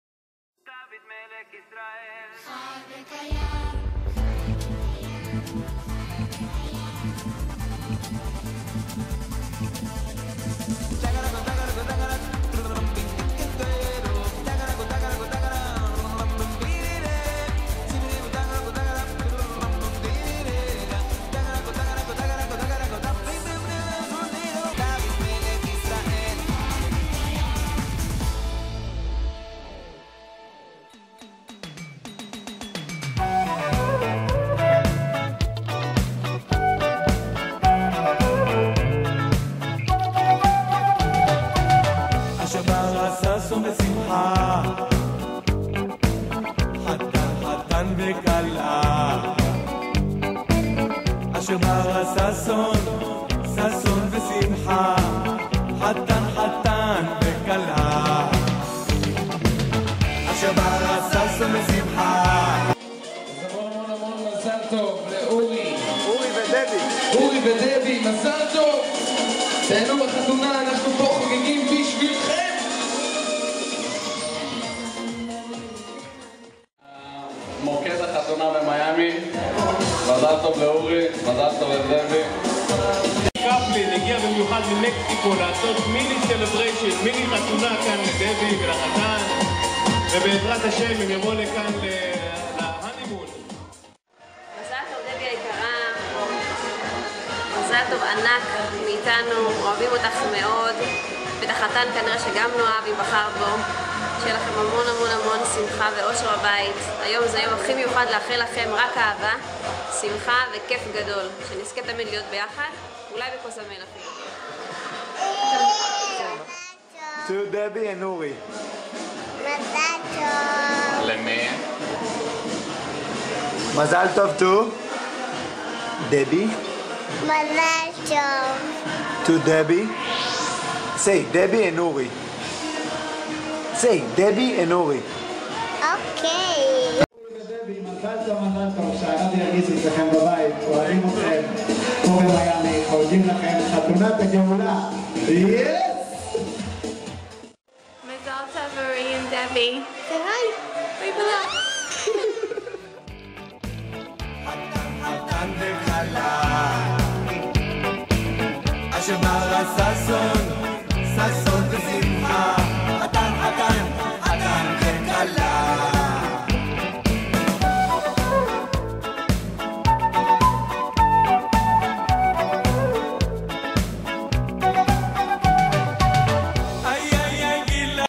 the Wedding in Miami חתונת הגאולה במיאמי
חב"ד קוזומל ומשפחת ישראל ממיאמי חוגגים (מפני שנבצר מהם להגיע לארץ) לאורי ודבי שמתחתנים השבוע בארץ

מזל טוב!